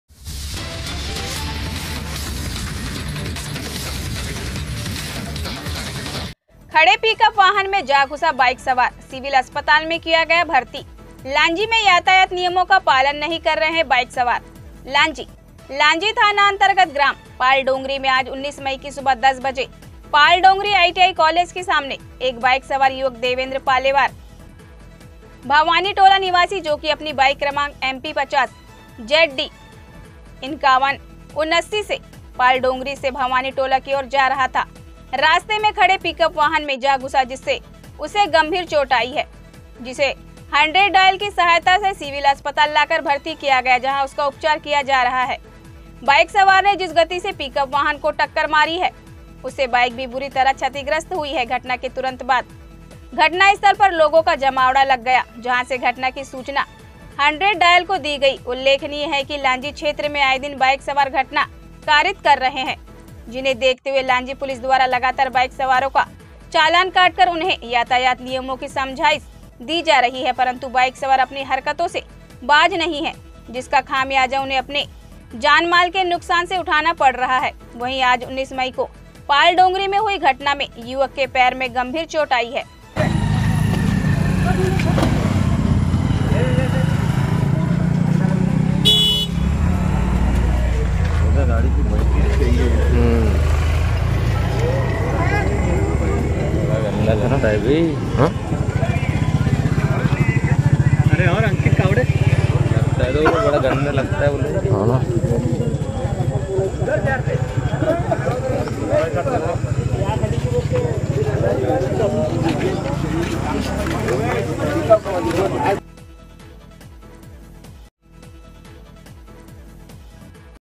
खड़े पिकअप वाहन में जा घुसा बाइक सवार, सिविल अस्पताल में किया गया भर्ती, लांजी में यातायात व्यवस्था,
*खड़े पिकअप वाहन में जा घुसा बाइक सवार, सिविल अस्पताल में किया गया भर्ती*

*लांजी से रिपोर्टर पवन कश्यप की रिपोर्ट ।।*